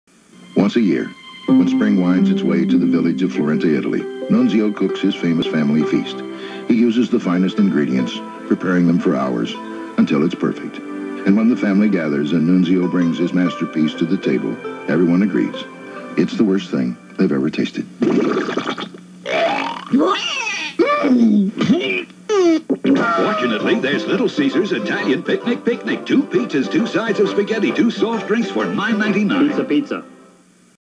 Hilarious LITTLE CAESARS commercial "Nunzio's Feast," Italian Picnic Picnic, ABC/WHAS, July 22 1993
Even flies and stone statues can't deal with Nunzio's cooking. A funny parody of those snoozy "tradition"-type ads of the early 1990s, with the same serious, homey narrator. I worked on the sound a little bit. Aired during part 5 of The Thorn Birds, on ABC/WHAS Louisville, Kentucky.

From the Outer Galaxy Lounge media archives; Mom's VHS collection.